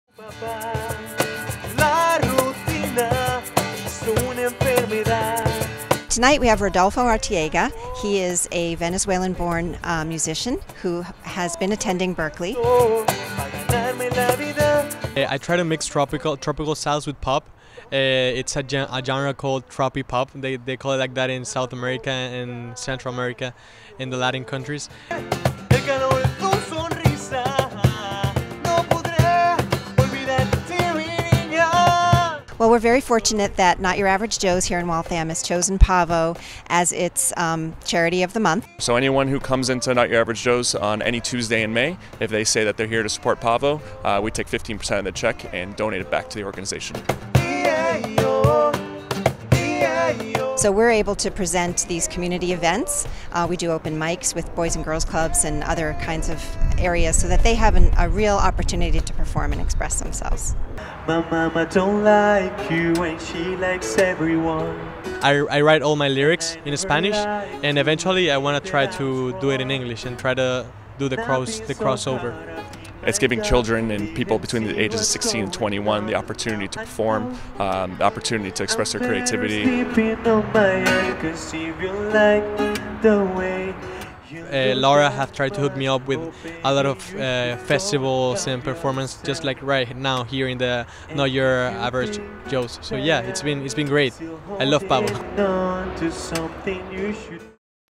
NYAJ Joins Pavoh to Promote Young Artists
Waltham Newswatch Tuesday, May 31, 2016
A young singer/songwriter from Venezuela performs at Not Your Average Joe's in Waltham as part of a May concert series organized by Pavoh.org